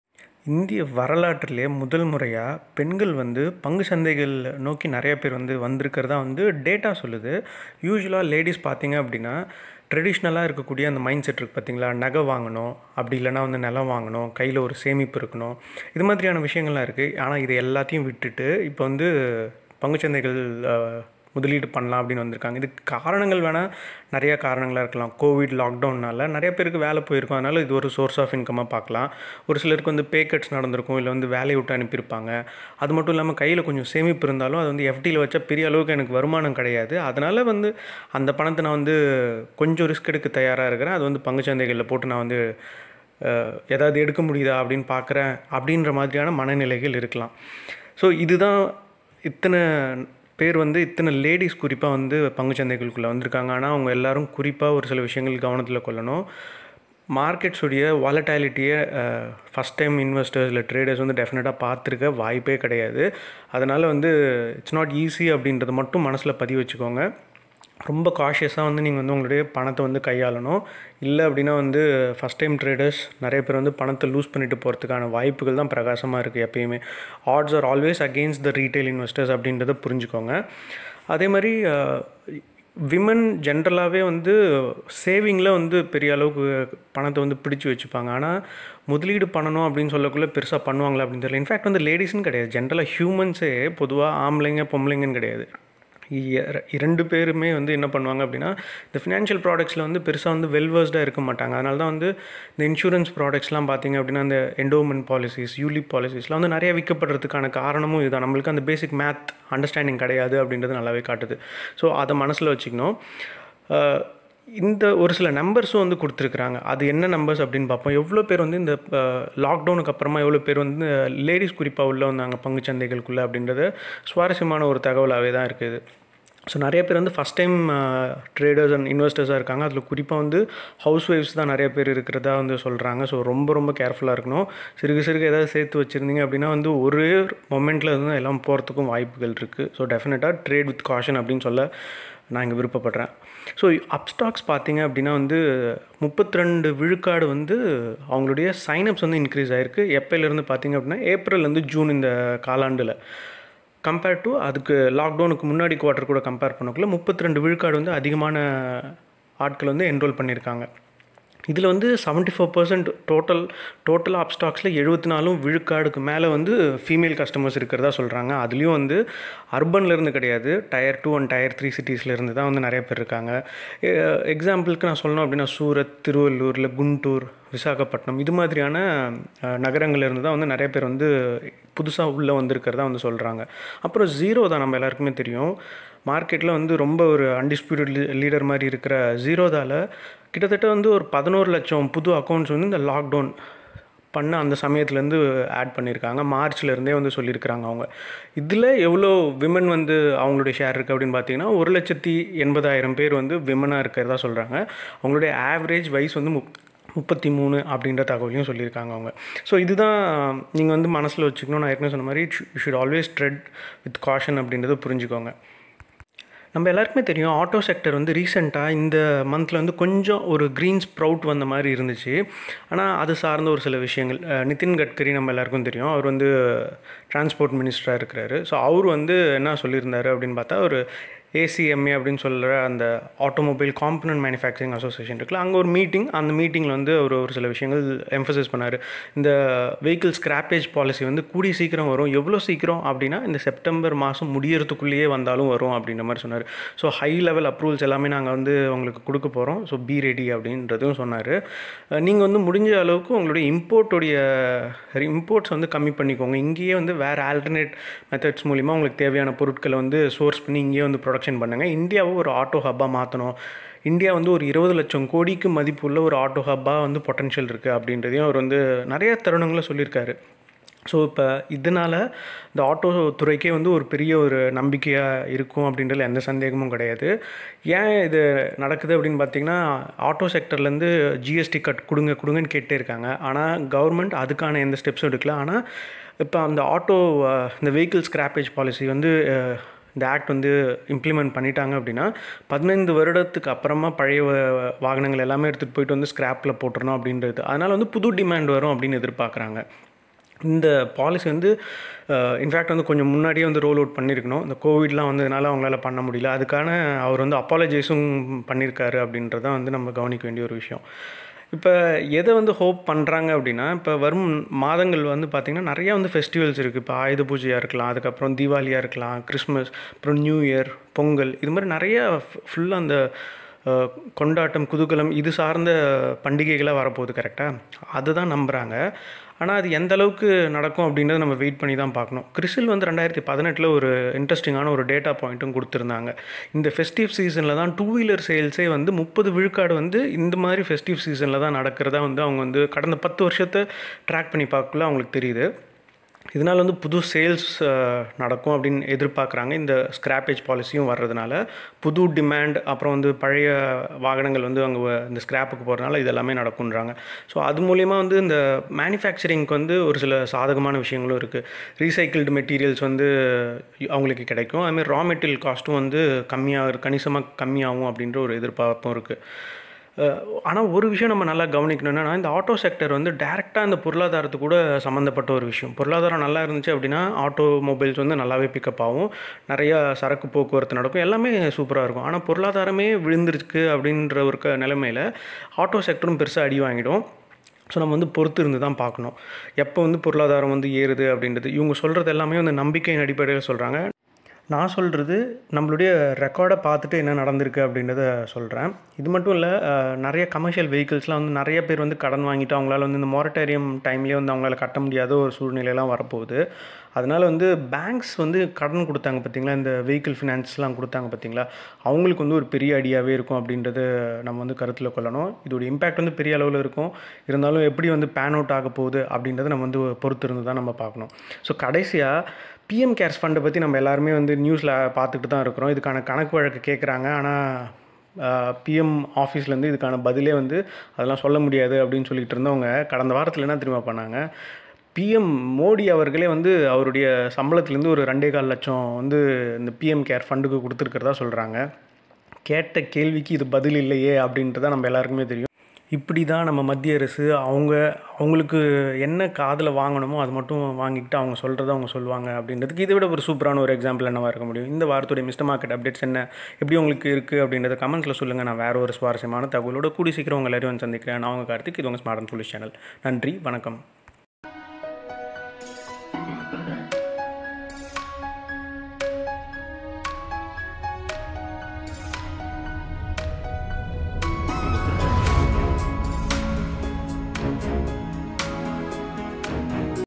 Mr.Market#22: Vehicle Scrappage Policy, More Women In Stock Market | Tamil
Auto Industry Tamil: Vehicle scrappage policy is expected to give the much needed boost to the auto sector. Nitin Gadkari has said, this policy will get implemented by the end of September 2020. The auto sector which has been asking for a GST cut, is hoping to ride on the new policy.

If implemented successfully, there will be a pent-up demand for the auto sector.